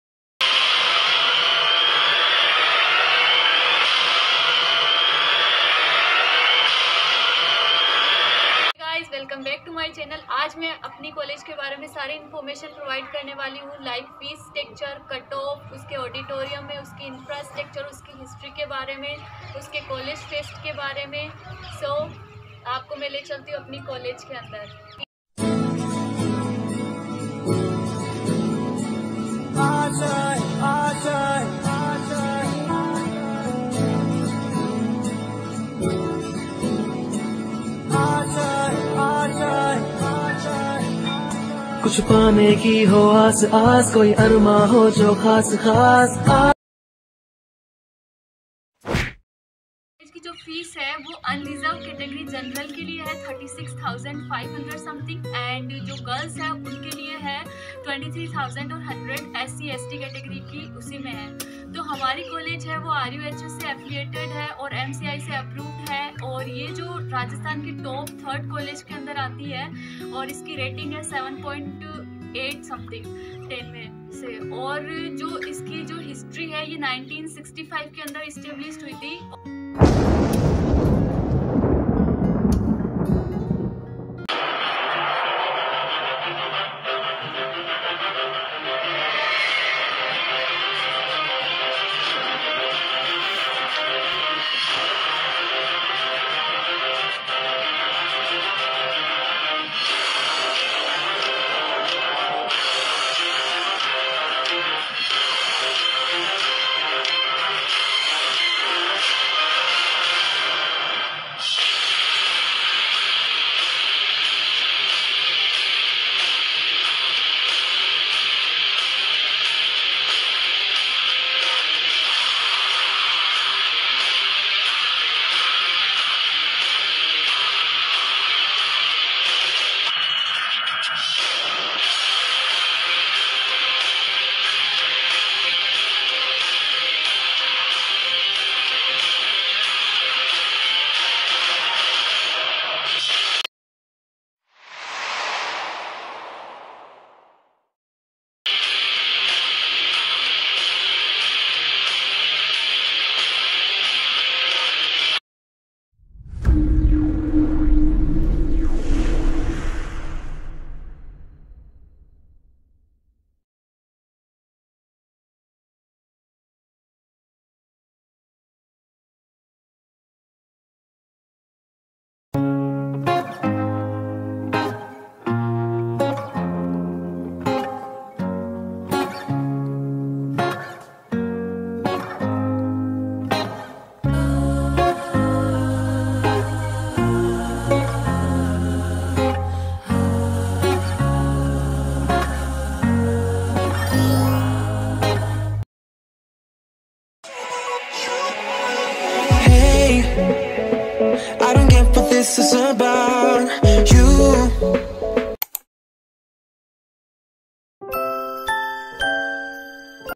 All about Dr.snmc jodhpur - fee,cutt off!!(.part -1)neet
Dr snmc jodhpur
1 fee structure
2.infrastucture
3 auditorium ...

Cutt off.